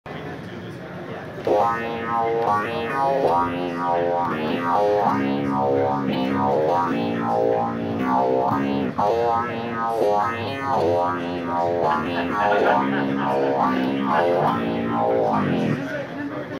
Maxime Graf bass guitar to MIDI on MicroKORG
Bassist Maxime Graf demonstrates the accurate tracking and fast latency of our audio-to-MIDI technology in the Product Showcase at AES 2019 in Dublin, Ireland.